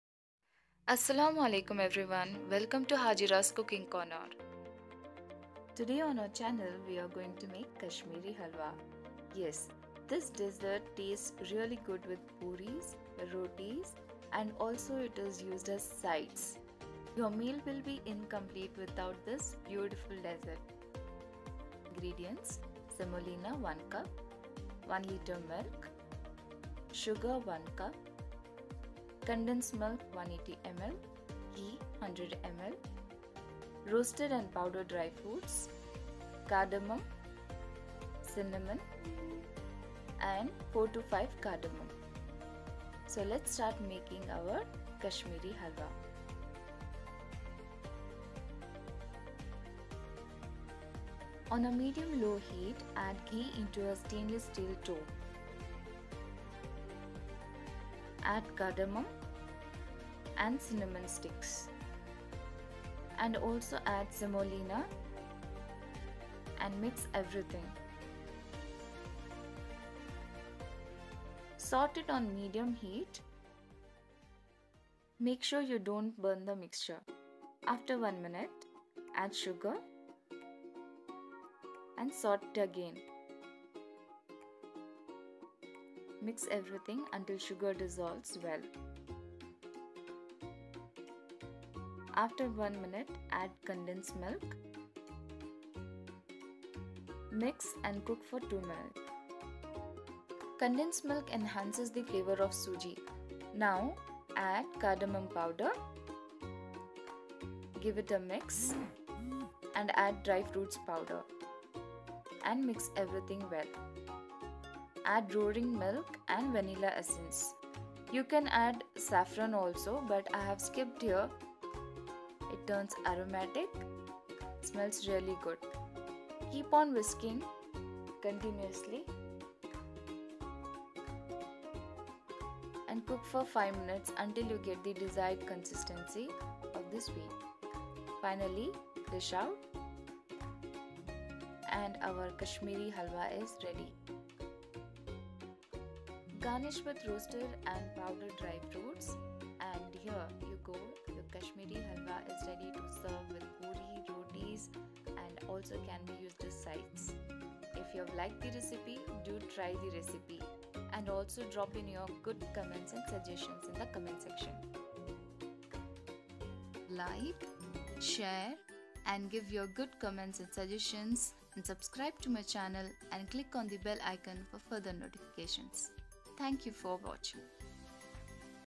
Kashmiri Halwa easy to make in festive seasons
Festive season is around and this sweet completes your sweet taste buds. Easy to make and delicious in taste 😋